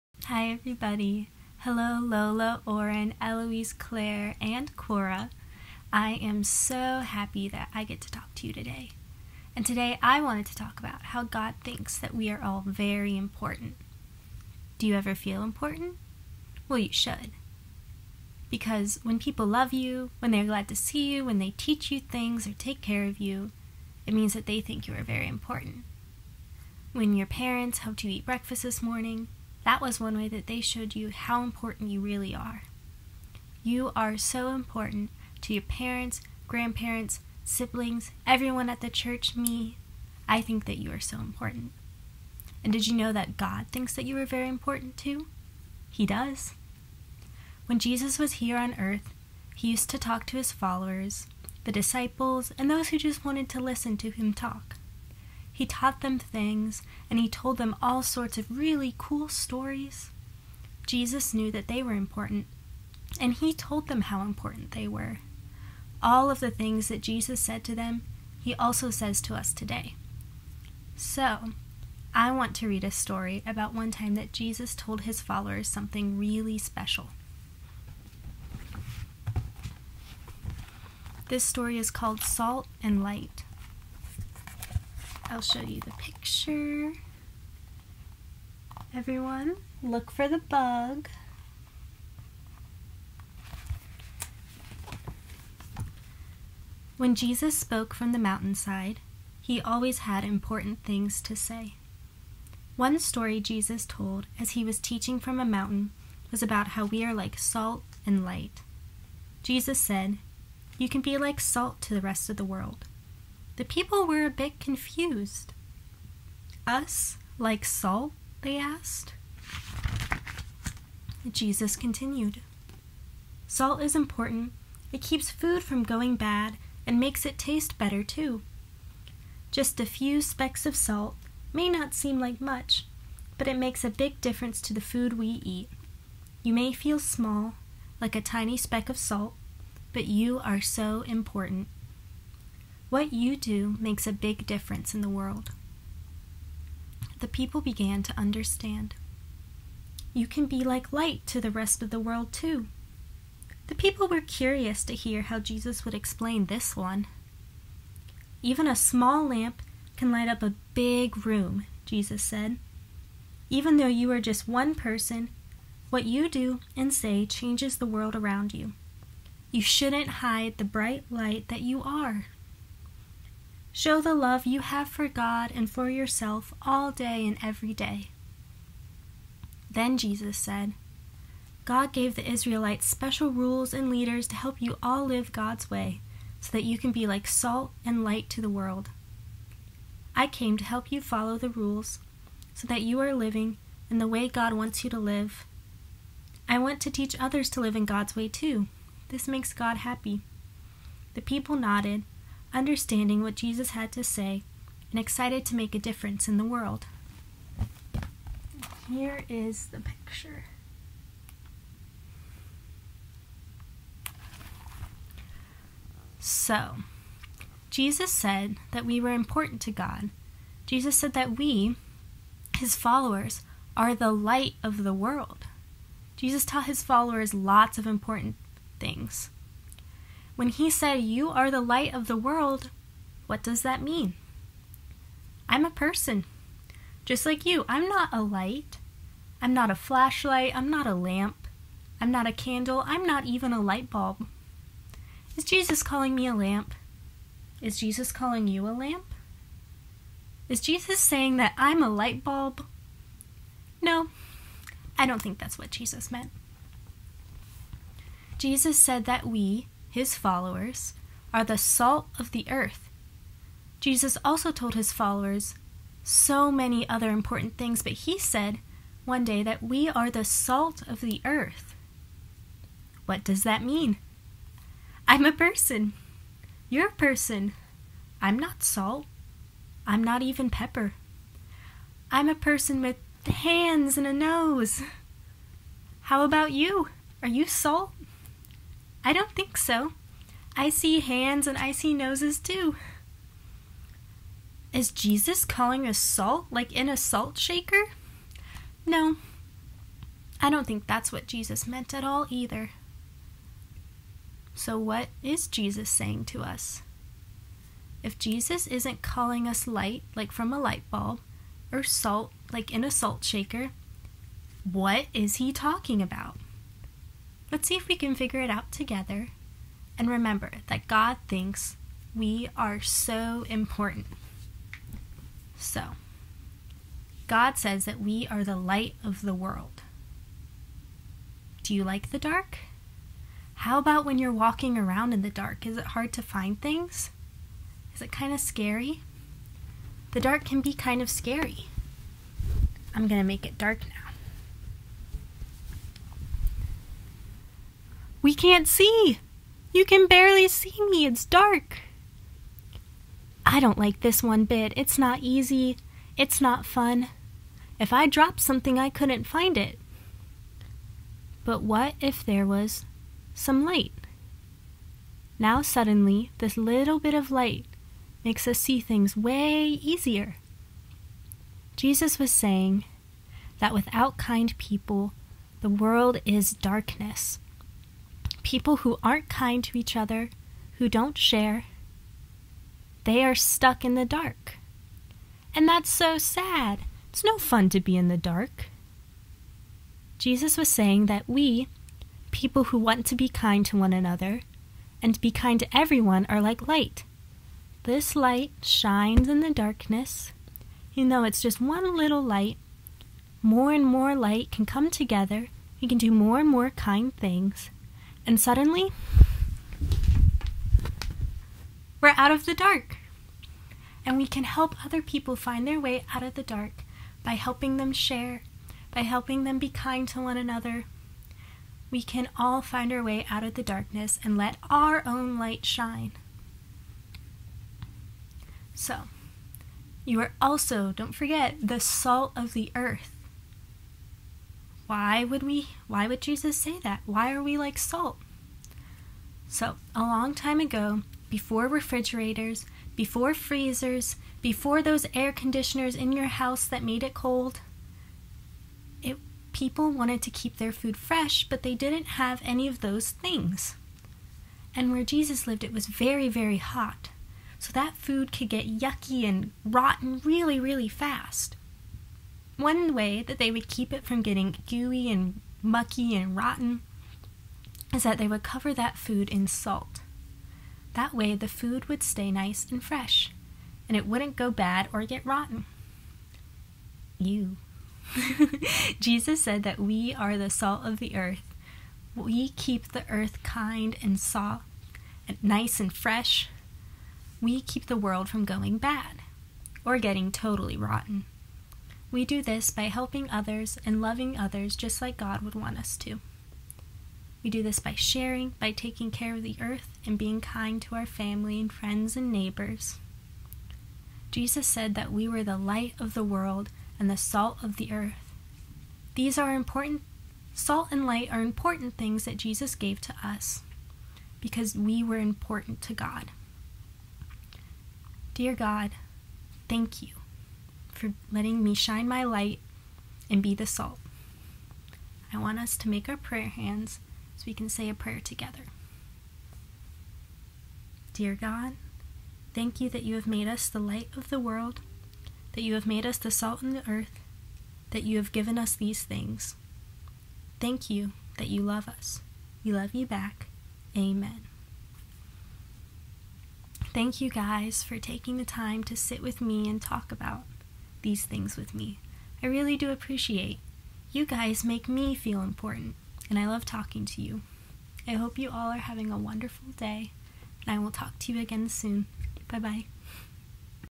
Sunday Preschool with Rhonda Krehbiel - Sunday, May 17, 2020
Did you know that you are very important to God?  Rhonda teaches us what it means to be the salt and the light of the earth...